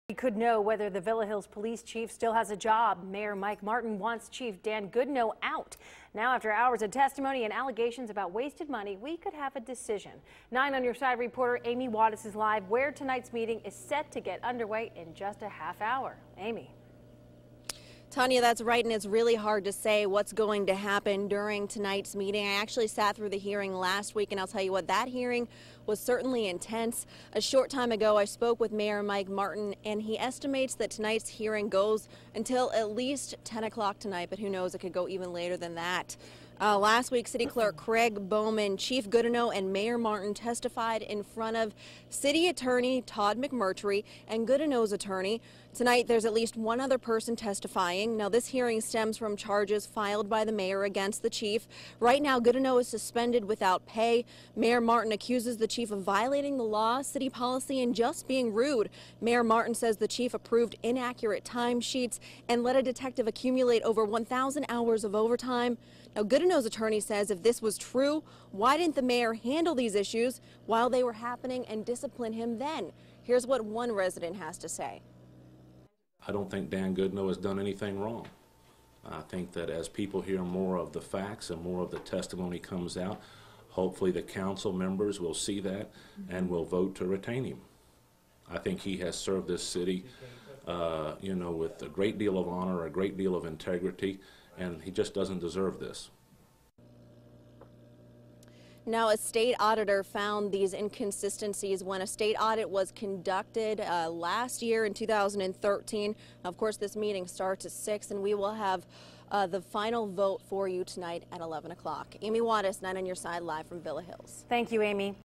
Future of Villa Hills police chief
Decision could come tonight; Mayor wants chief out